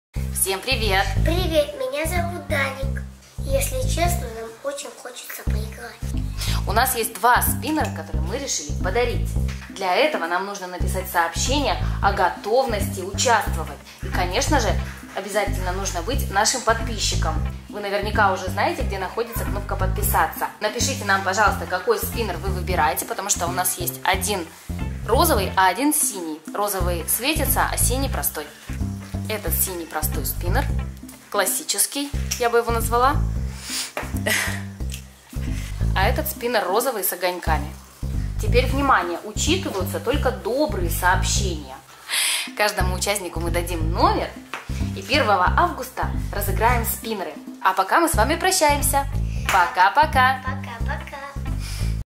Срочно! Дарим спиннеры! Заявки до 30 июля! Розыгрыш 1 августа!
Для участия нужно:
-оставить добрый комментарий
-написать какой спиннер вы хотите

После того розыгрыша 1августа, я напишу электронный адрес, куда присылать адрес людям, чей номер выпал.